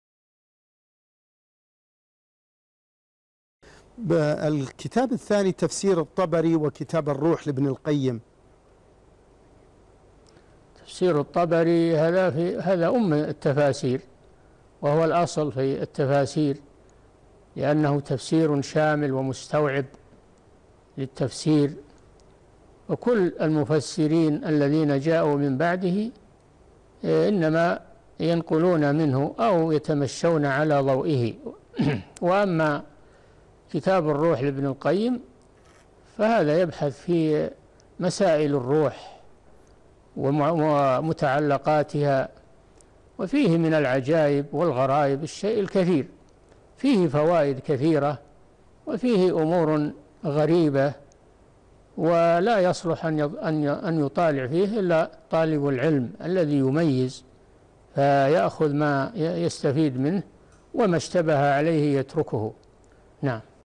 يسأل عن كتاب تفسير الطبري وكتاب الروح لابن القيم - الشيخ صالح الفوزان
نص السؤال: أسأل عن كتاب تفسير الطبري وكتاب الروح لابن القيم؟ رابط الحلقة كاملة
السيرة الذاتية :
للاشتراك في قناة الشيخ صالح الفوزان
لمشاهدة حلقات فتاوى على الهواء كاملة

رئيس الفريق
لمشروع كبار العلماء